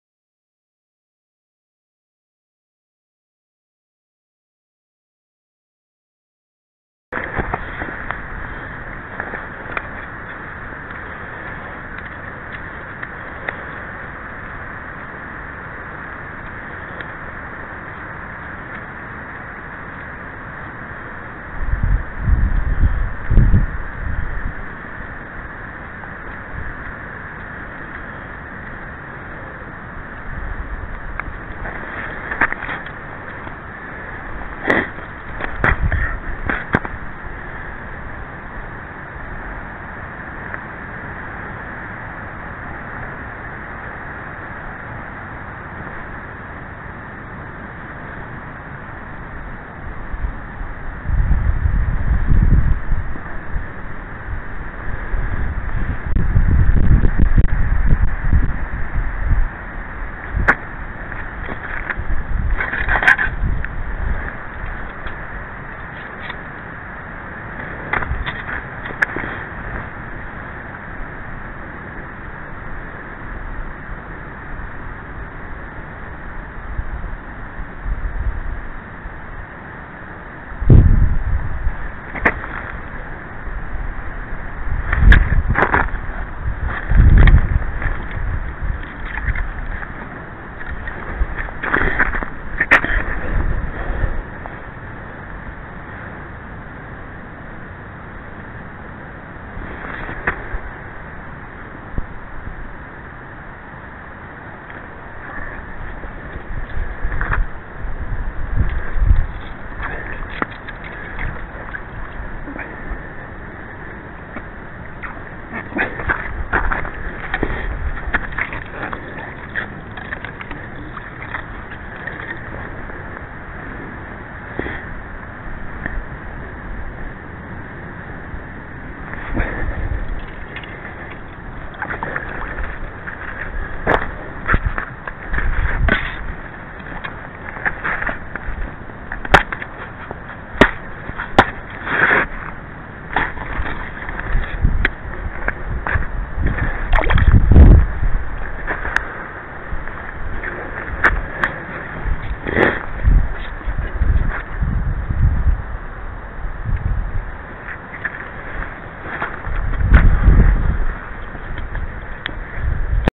Hauki 28.2.2019
Metsälammen melkein viimonen hauki alkukeväällä 2019. Nyt on siialla hyvä elää. ???